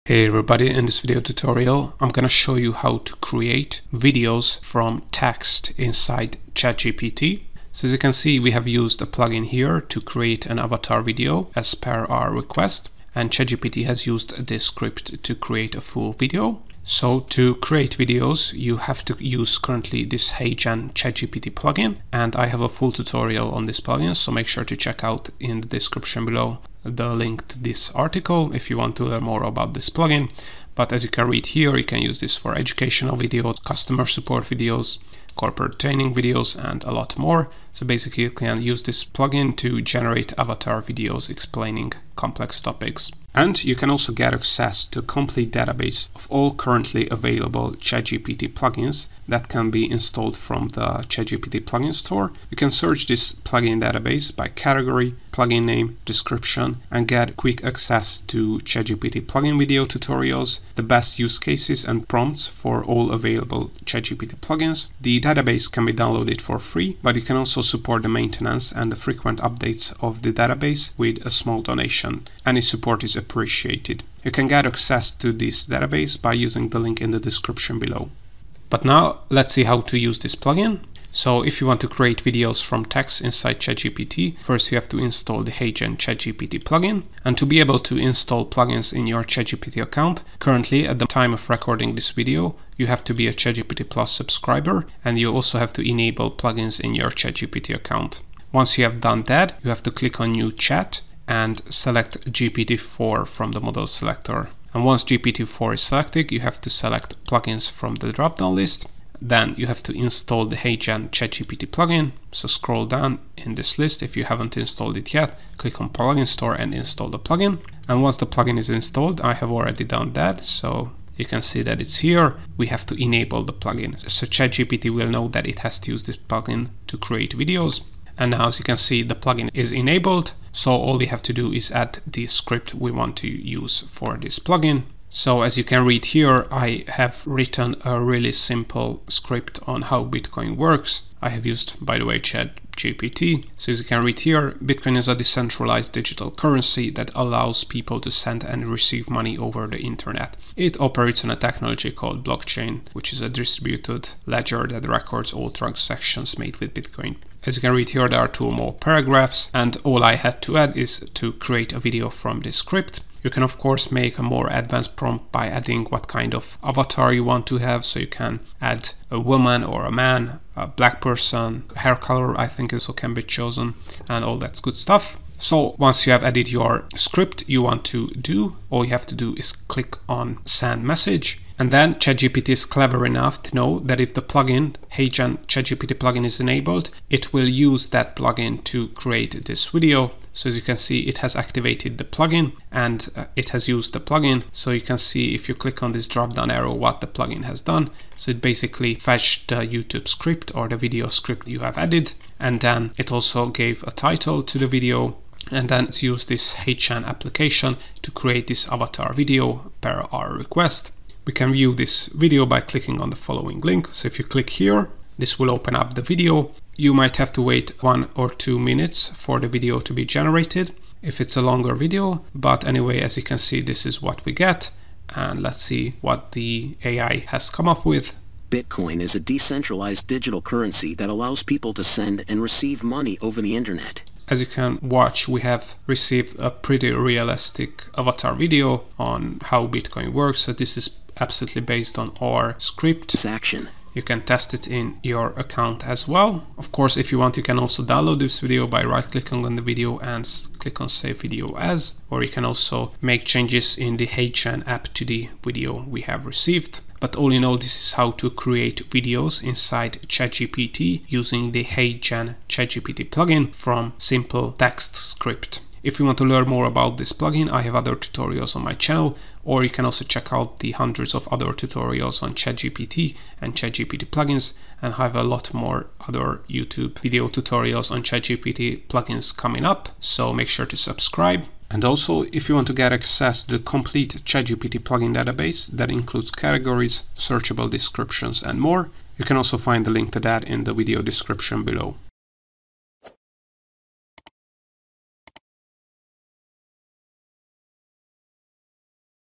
How To Create Videos Inside ChatGPT? (Text To Video With ChatGPT)
. - Learn how to create YouTube videos inside ChatGPT using a text to video with ChatGPT plugin.